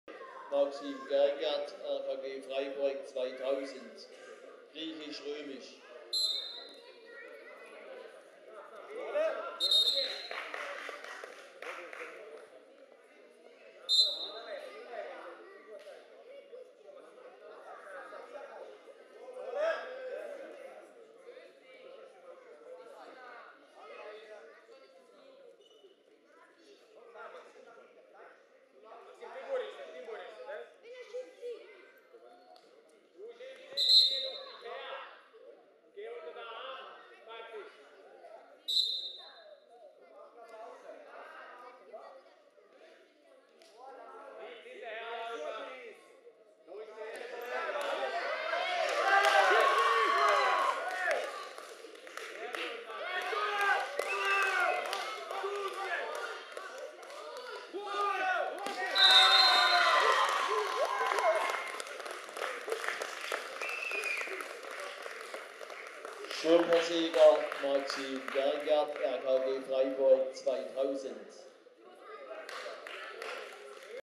RKG-Video 005-151219: Maxim Gergert
Bezirksjugendliga Breisgau-Ortenau -
Hannes Gunkel (ASC Kappel J / rot) vs Maxim Gergert (RKG Freiburg 2000 J / blau)